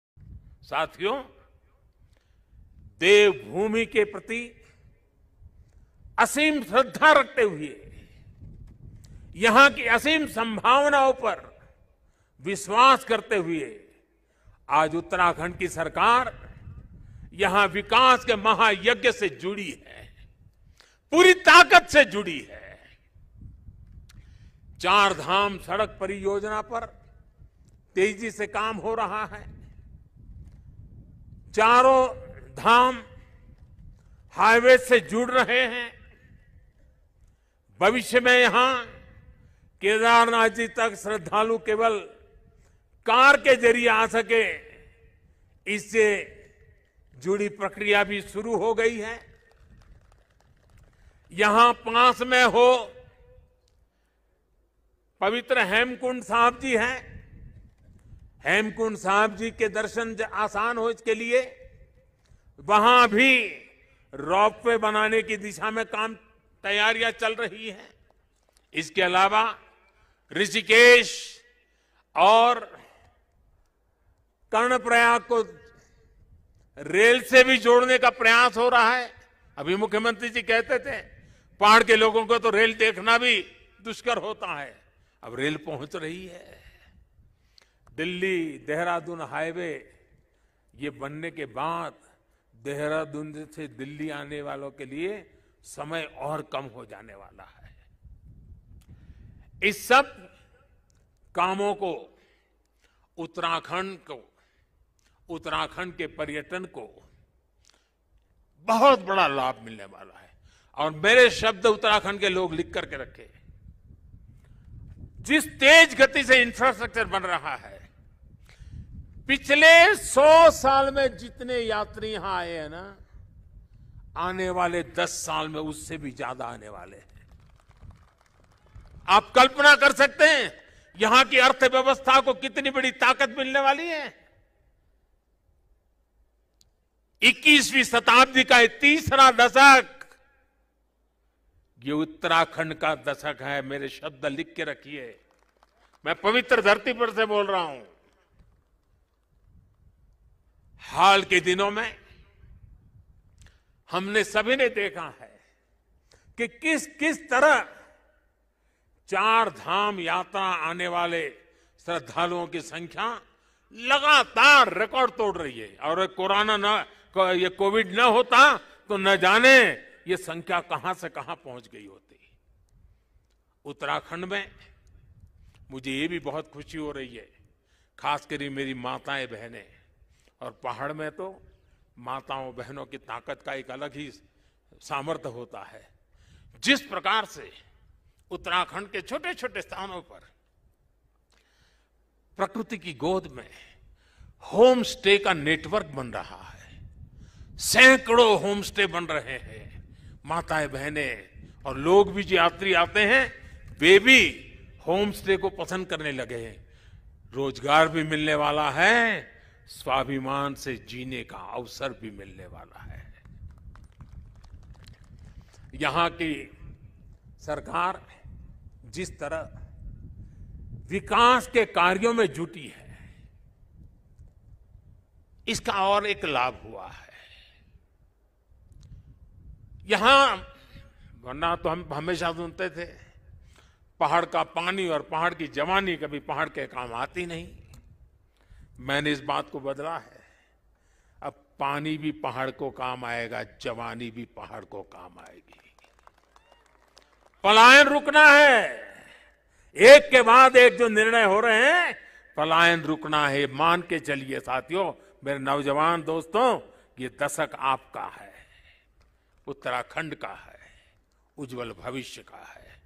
PM Modi Kedarnath Speech: जितनी ऊंचाइयों पर उत्तराखंड बसा है, उससे भी ज्यादा ऊंचाइयों को हासिल करेगा
जितनी ऊंचाइयों पर उत्तराखंड बसा है, उससे भी ज्यादा ऊंचाइयों को हासिल करेगा।